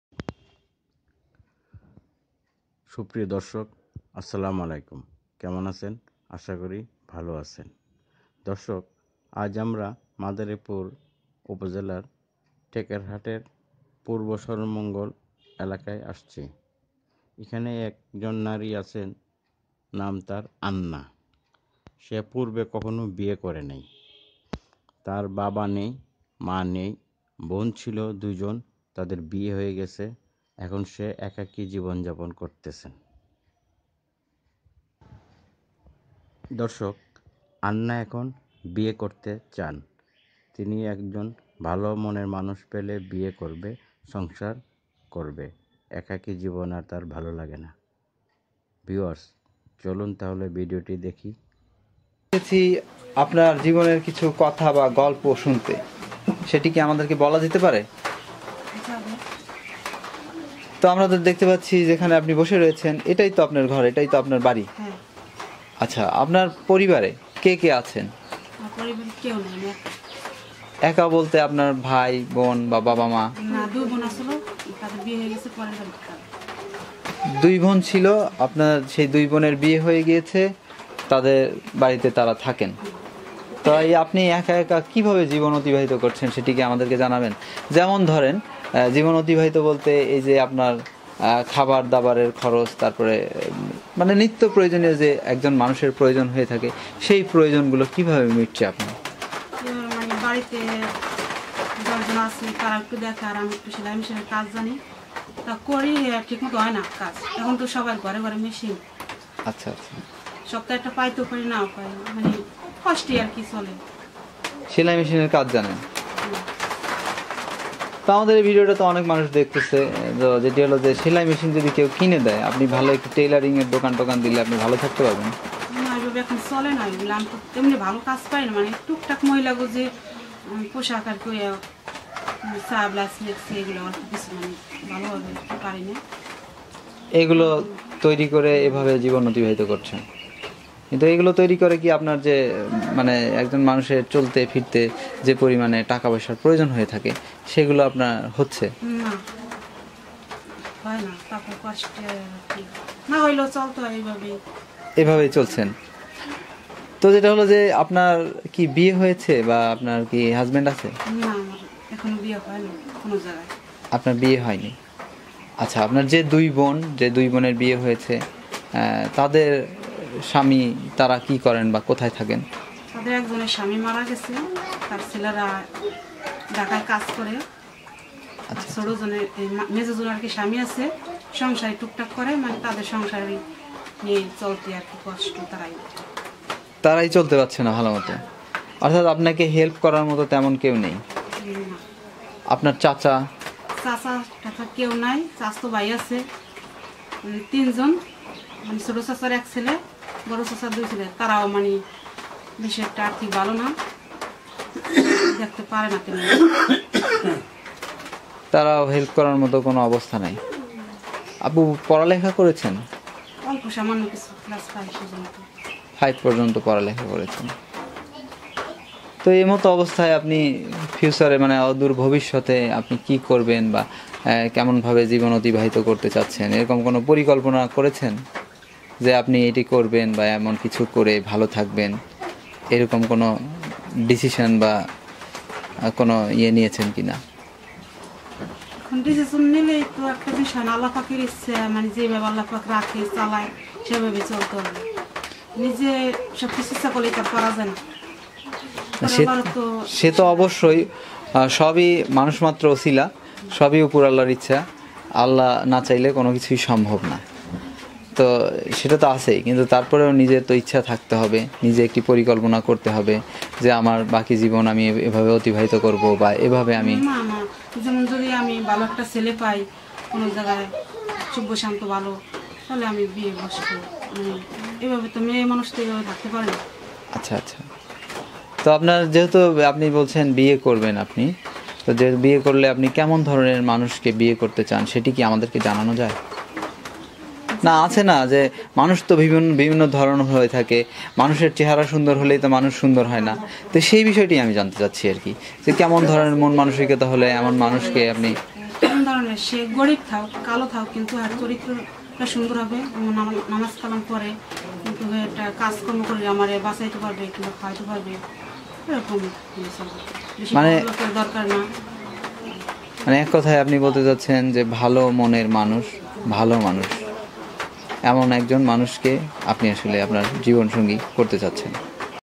Life history/আন্নার বিয়ে হয়নি কেন/ কাকে বিয়ে করে আন্না সংসার করতে চাই জানতে দেখুন
দর্শক আমরা এই ভিডিওটি ধারণ করেছি মাদারীপুর টেকেরহাট এর পূর্ব স্বরমঙ্গল এলাকা থেকে। এই নারীর কেউ নেই মা নেই বাবা নেই দুই বোন ছিল তাদের বিয়ে হয়ে গেছে সে আগে বিয়েও করেনি একাকী জীবন যাপন করতেছেন। আন্না এখন বিয়ে করতে চায়।

@asbbanglatv# আন্নার জীবনের গল্প#life style###